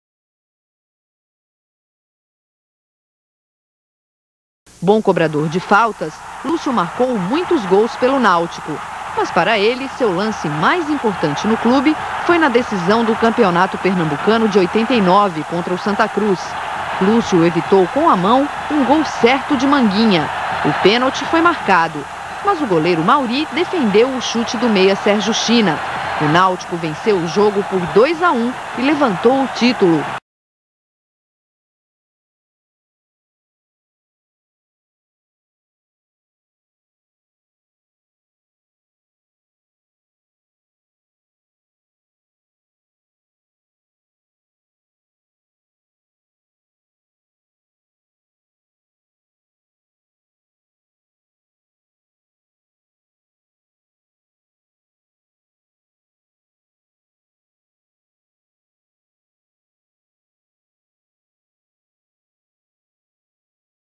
Final de 1989: A mãozinha do zagueiro Lúcio Surubim
Em 1989, o Santa Cruz perde o título Pernambucano com mãozinha do árbitro e do zagueiro Lúcio Surubim

Ficha do Jogo:

Náutico 2 x 1 Santa Cruz
Data: 03/08/89
Terceiro Jogo da Decisão: Náutico - Campeão 1989
Local: Estádio do Arruda
Gols: Erasmo e Bizú (Náutico); Marcelo Rocha (Santa Cruz)
Árbitro: João José Venceslau
Renda: NCz$ 59.320,00
Público: 11.134 pagantes

Náutico: Mauri; Levi, Lúcio Surubim, Romildo e Júnior; Muller, Erasmo (Vavá) e Haroldo; Newton, Bizu e Nivaldo.
Técnico: Charles Muniz

Santa Cruz: Waldir Peres; Eduardo (Ragner), Marcão, Orlando e Lotti; Sales, Ataíde e Sérgio China; Leto (Marcelo Rocha), Manguinha e Orlando
Técnico: Givanildo Oliveira